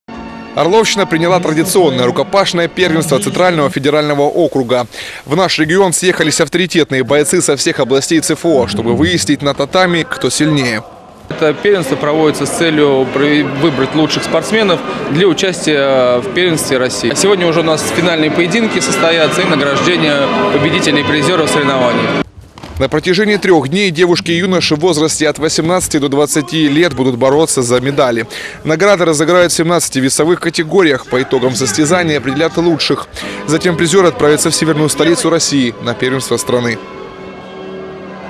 Рукопашный бой
Орловщина приняла традиционное рукопашное первенство Центрального федерального округа. В наш регион съехались авторитетные бойцы со всех областей ЦФО.